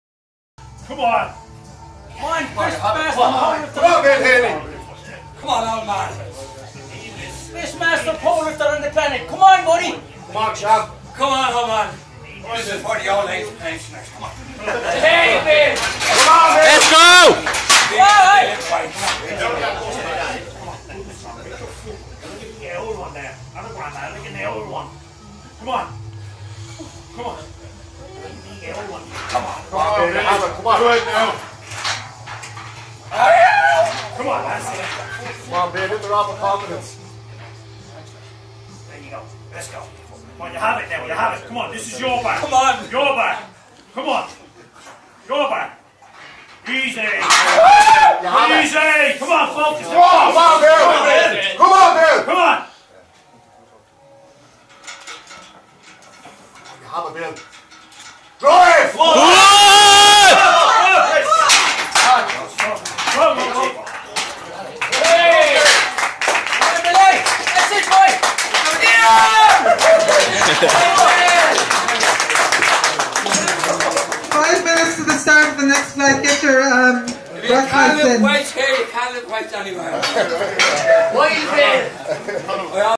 Bill Healy (Dublin) squat 2016
Bill Healy 66 years 230 KGS masters world record (Patrickswell Limerick) 2016